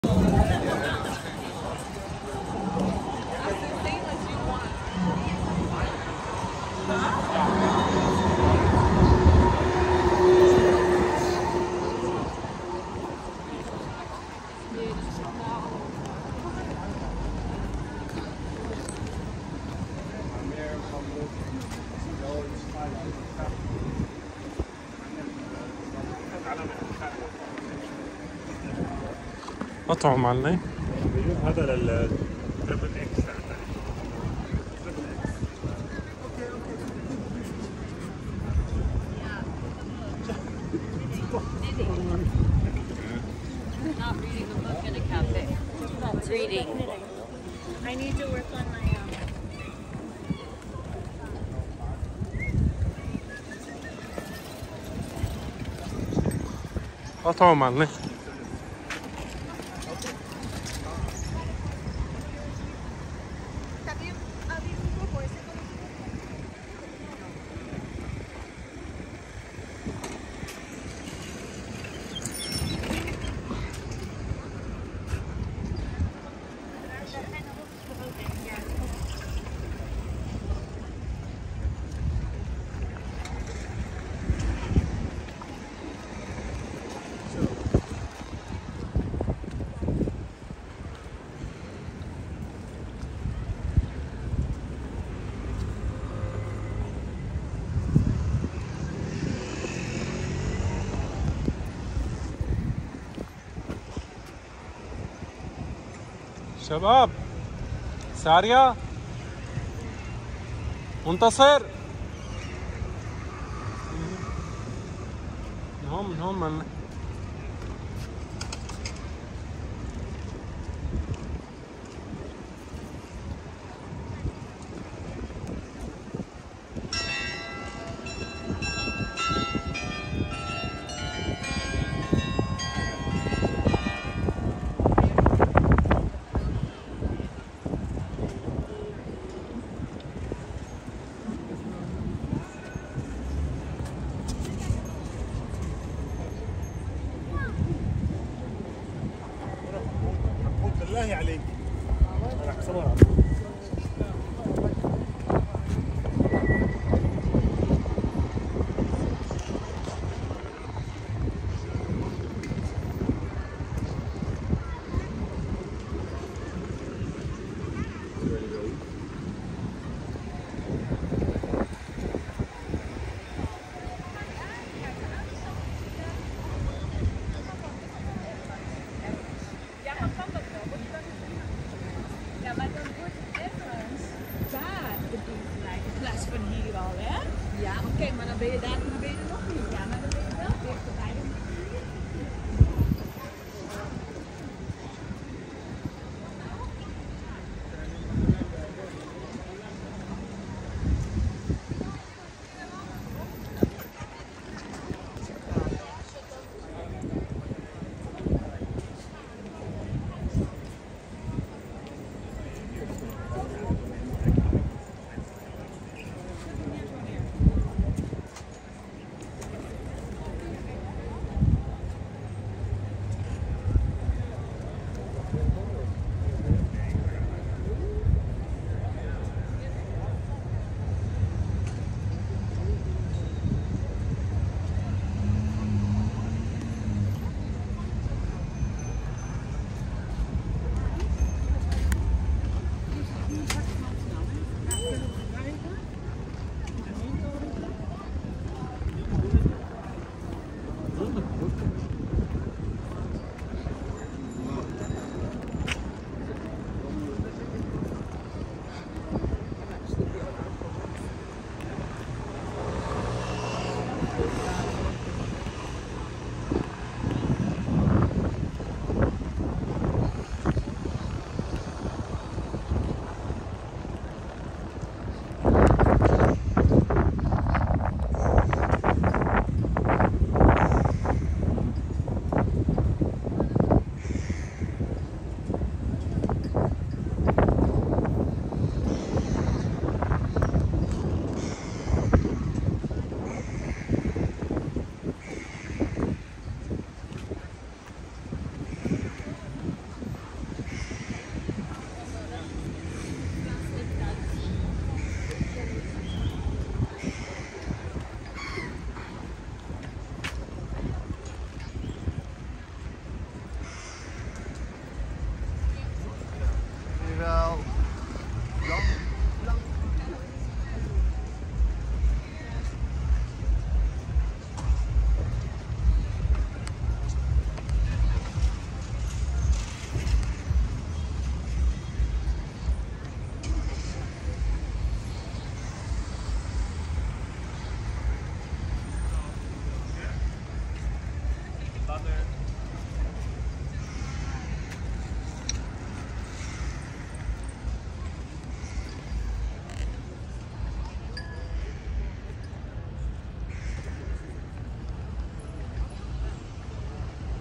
Amsterdam, NL: Ep.3.2 Muntplein Square [4K]
Series(9): A Complete Tour in Amsterdam, The Netherlands.
Stay with us for realistic and complete tours in the most beautiful touristic cities in the world.